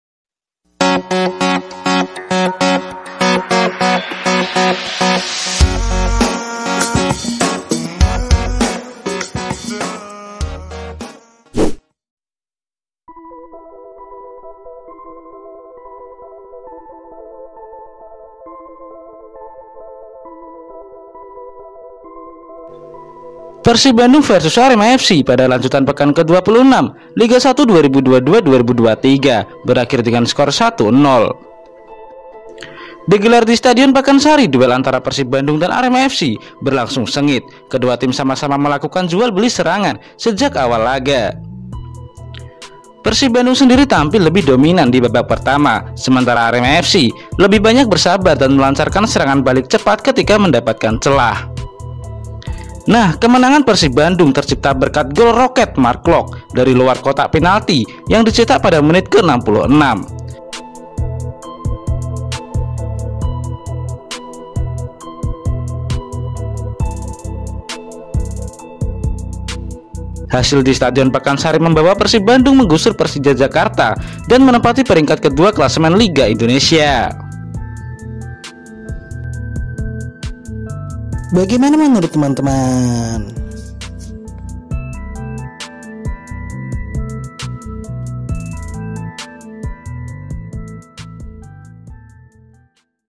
Bikin AREMA FC Kalah Lagi‼️ Gol Berhias Roket Marc Klok Bawa PERSIB Menang
Ikuti terus update video terbarunya..